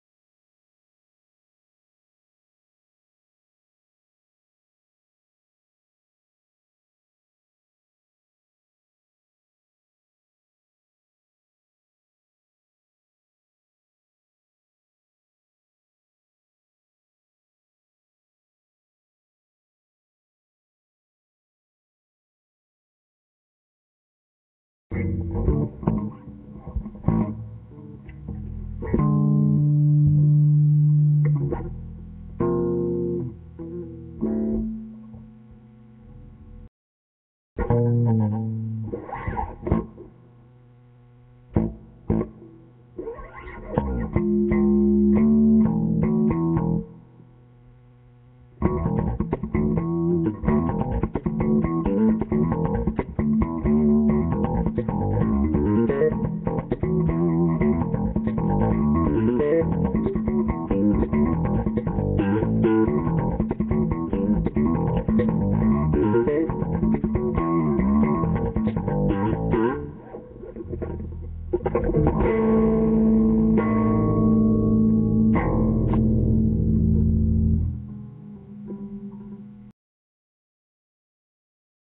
First audio test
Bass from Mac Pro audio from iMac. This is a test for the first annual +Christopher Maute bass annihilation competition, to see who is the besets bass player out of anyone ever.  LOL not really can you imagine though.